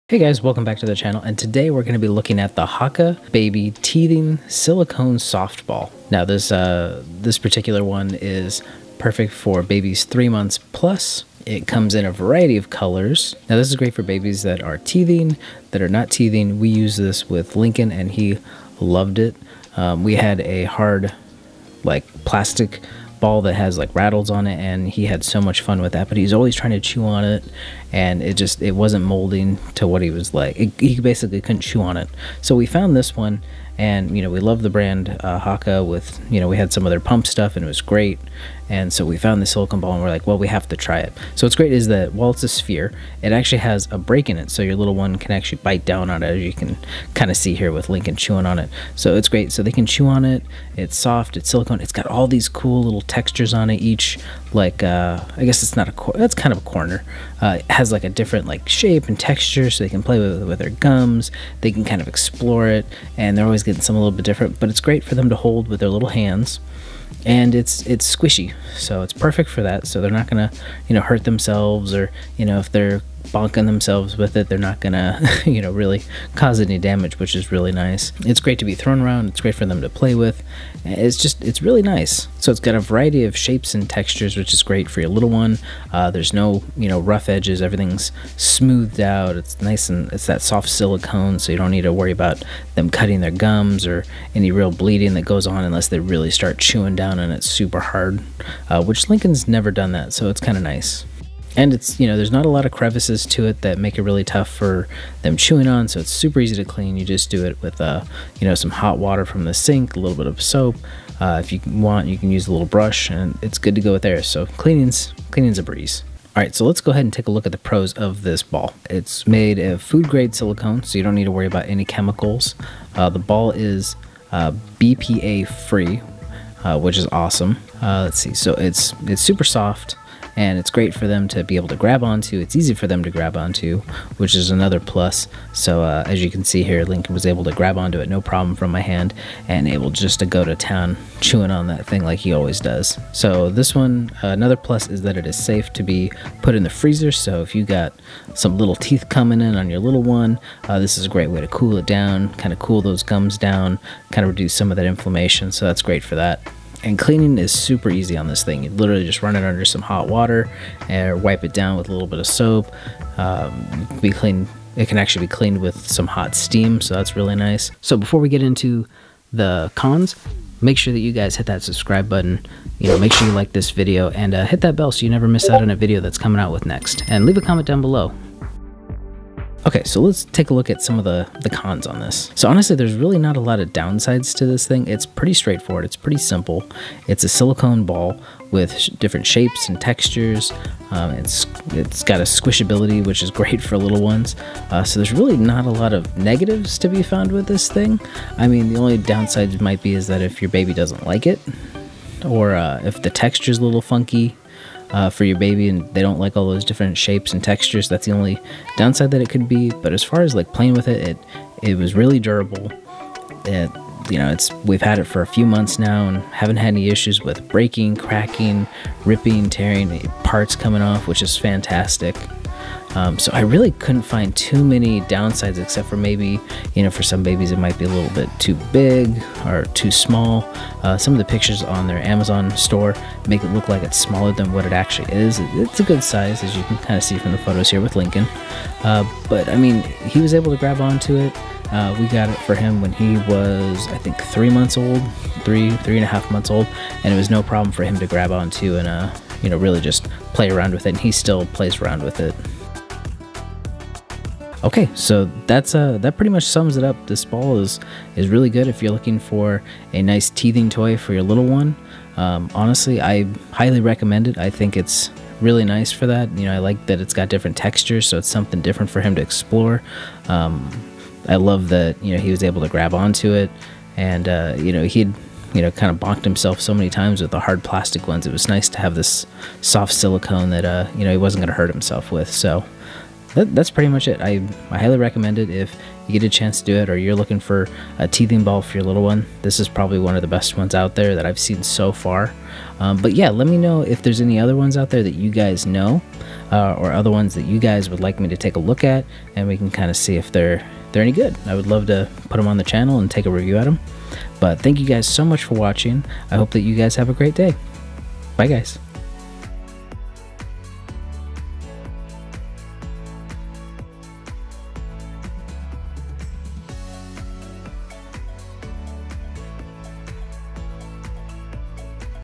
Haakaa Silicone Teething Ball Review | Alexander the Dad
Time to chew! This Haakaa silicone teething ball is perfect for the little one to chew and play with. Let's take a look and review the pros and cons of this bouncing baby ball.

Ready to grab it?

Dimensions: Approx. 4 inches or 10cm diameter

Highlights:
100% food grade silicone
Soft, flexible and durable
Raised ridges for soothing teeth and gums
Easy to grab
Dishwasher and microwave sterilizer safe
Easy care
One-piece design with no joints, cracks or edges
BPA, PVC and phthalate-free